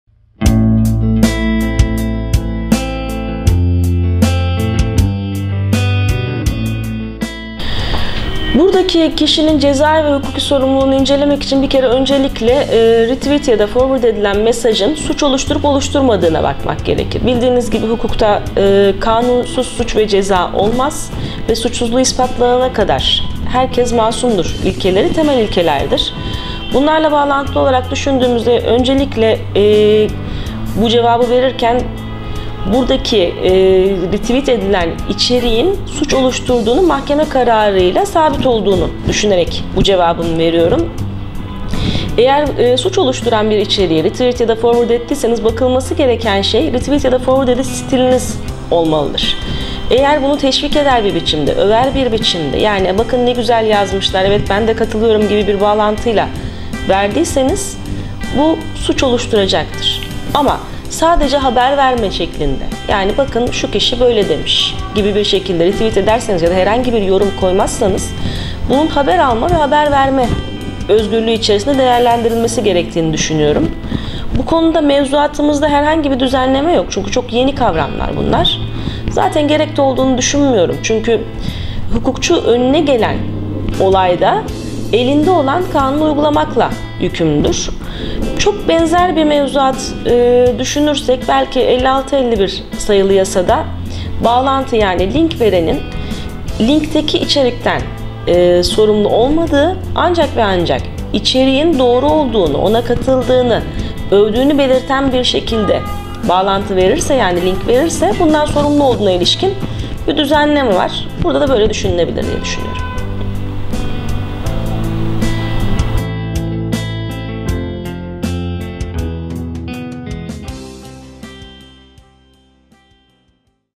AV.Ceyda Cimilli AKAYDIN [HukukTV.com]
Sosyal medyada bir başkasının oluşturmuş olduğu içeriği retweet/forward vb. yollarla ileten kişinin cezai ve hukuki sorumluluğu nedir?